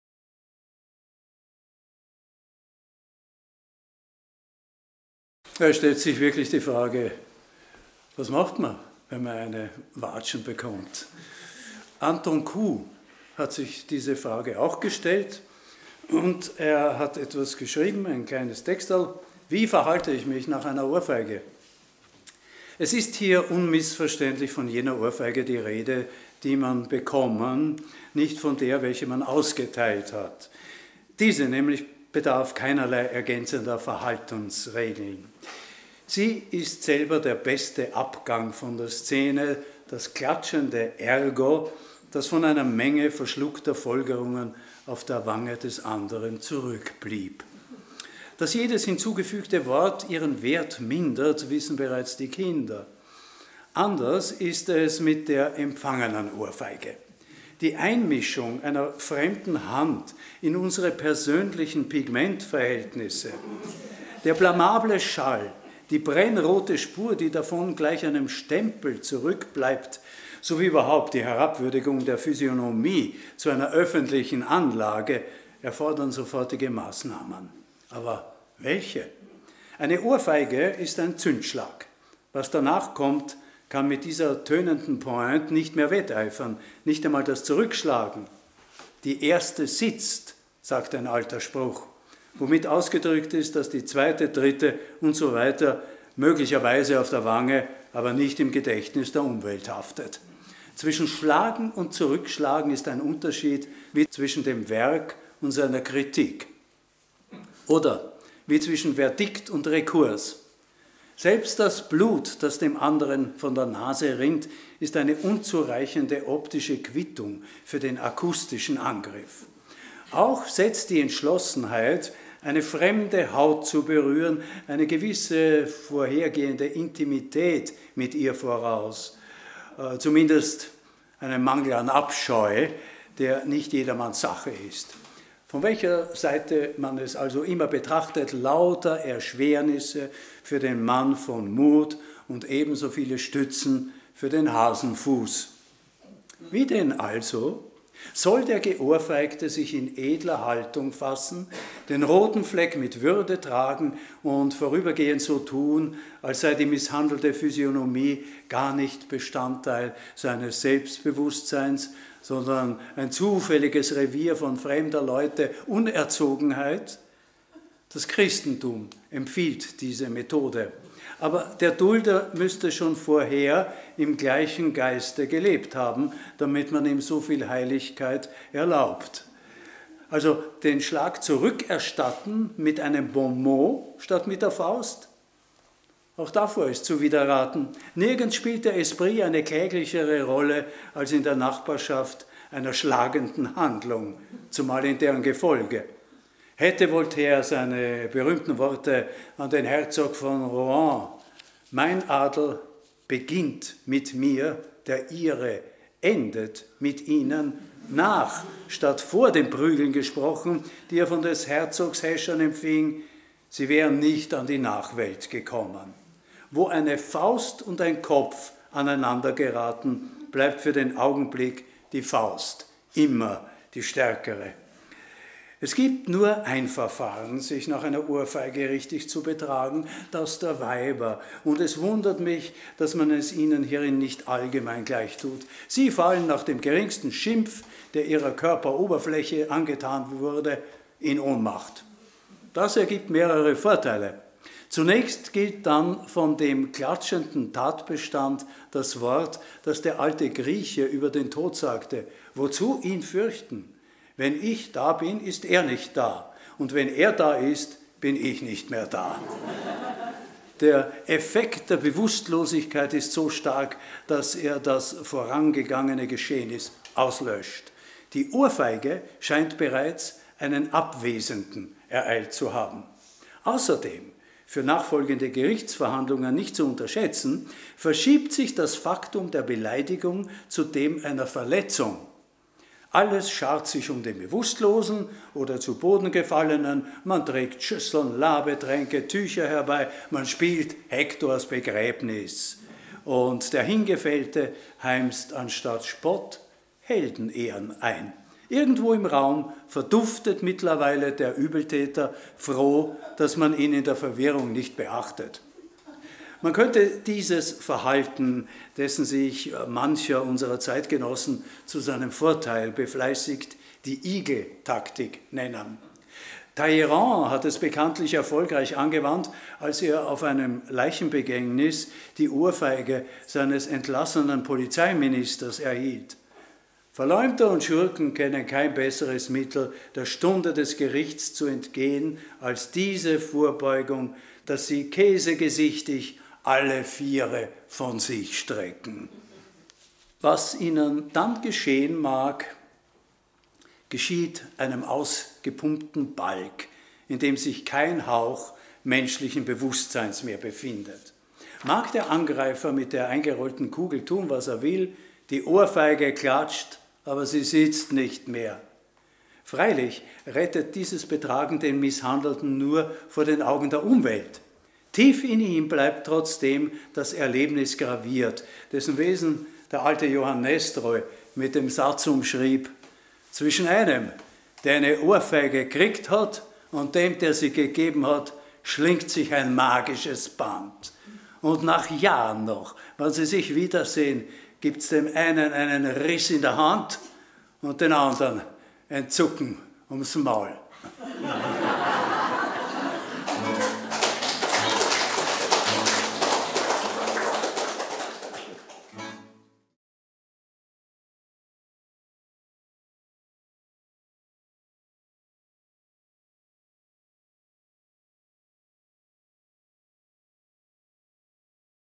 Anton Kuh "Wie verhalte ich mich nach einer Ohrfeige" (GELLERTS FEINE HÖRBÜCHEREI)
Satirische Texte von Anton Kuh (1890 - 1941) finden Sie immer wieder in GELLERTS FEINER HÖRBÜCHEREI, Texte, die Zeugnis davon ablegen, welch grossartiger Sprachkünstler der Schriftsteller und Stegreifredner war. Genial und witzig sind auch Kuhs Überlegungen zum Thema "Wie verhalte ich mich nach einer Ohrfeige"